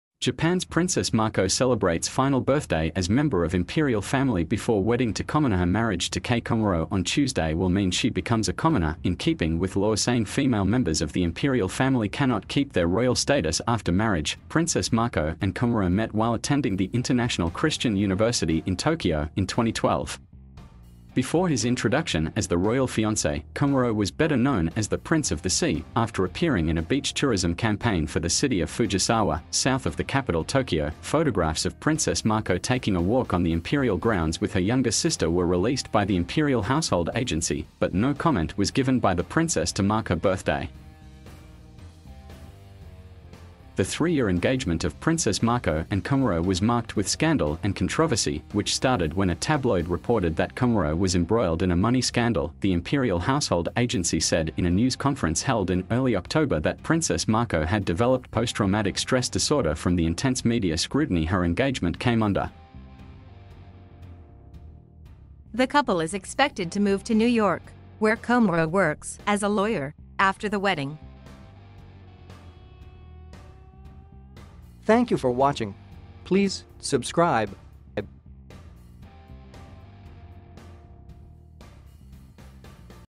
BREAKING NEWS! Japan's Princess Mako celebrates final birthday as member of imperial family bef
News of the world: BREAKING NEWS! Japan's Princess Mako celebrates final birthday as member of imperial family bef

Japan's Princess Mako celebrates final birthday as member of imperial family before wedding to commonerHer marriage to Kei Komuro on Tuesday will mean she becomes a commoner in keeping with laws saying female members of the imperial family cannot keep their royal status after marriage.Princess Mako and Komuro met while attending the International Christian University in Tokyo in 2012.Before his introduction as the royal fiance, Komuro was better known as the "Prince of the Sea" after appearing i

the-closed-door-of-trust.